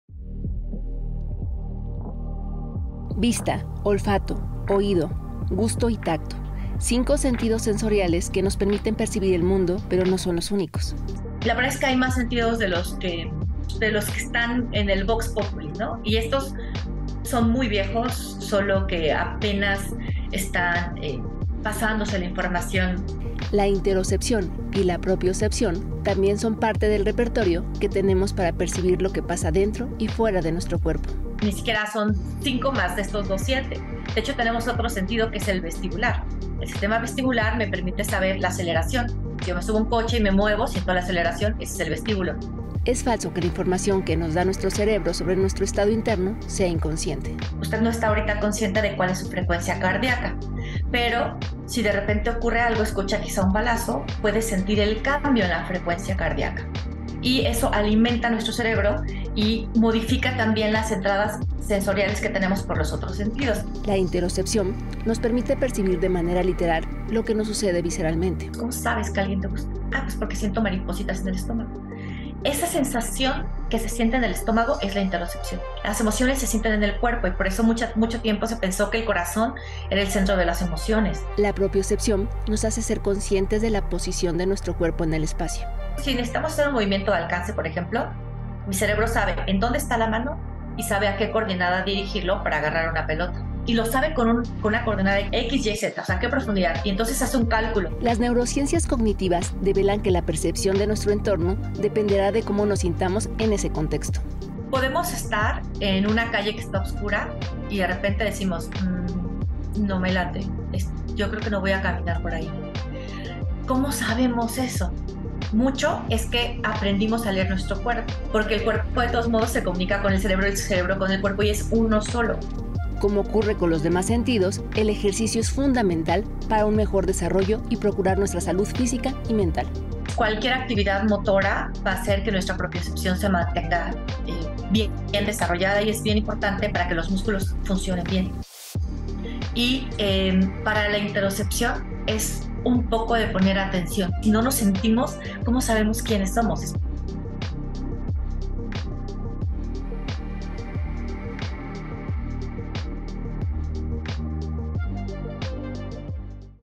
¿Mariposas en el estómago? Hay más de 5 sentidos
Oler, saborear, escuchar, mirar, tocar... Desde siempre nos han enseñado que hay 5 sentidos, pero en los últimos tiempos la neurociencia ha avanzado y surgieron otros dos: la interocepción y la propiocepción. ¿En qué consisten? Estos dos sentidos repercuten también en el estado de ánimo, la toma de decisiones y las funciones cognitivas.